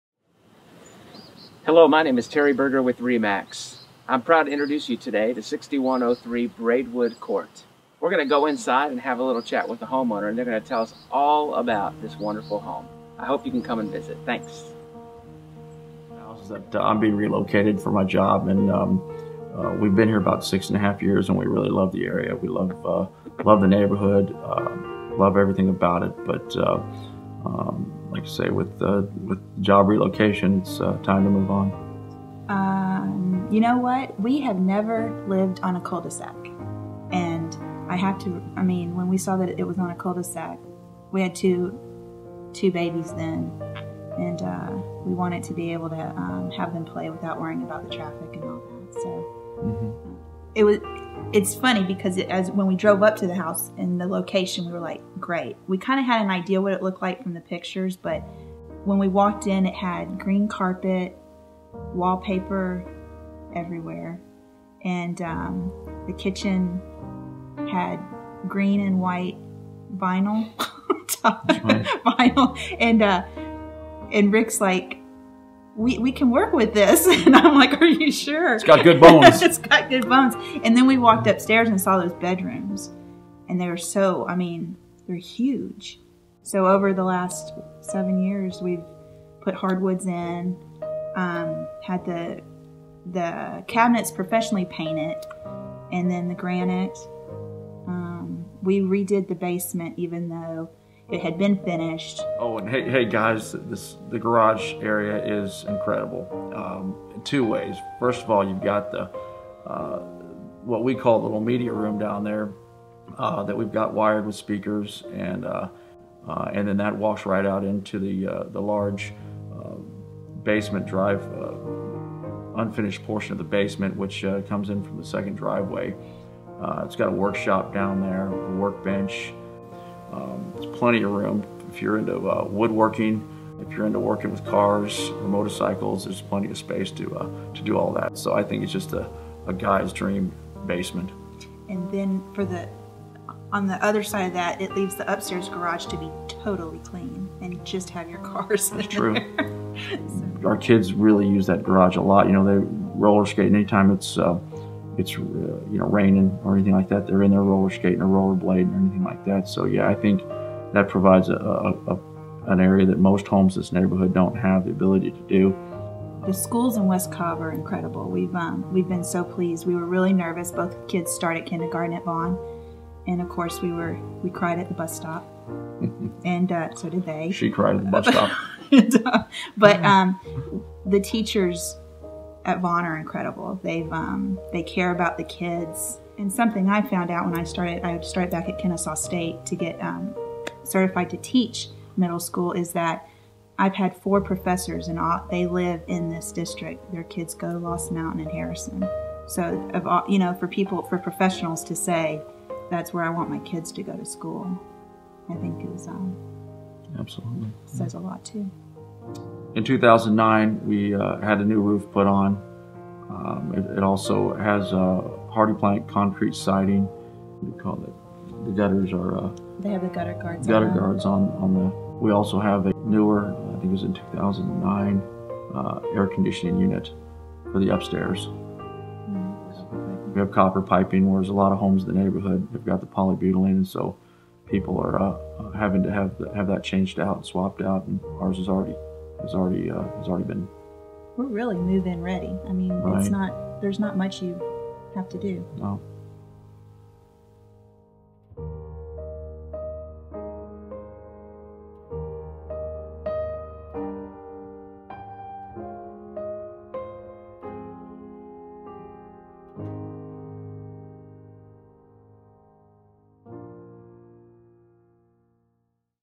Seller Interview - 6103 Braidwood Ct, Acworth GA - Brookstone Subdivision
An interview with the sellers of 6103 Braidwood Ct.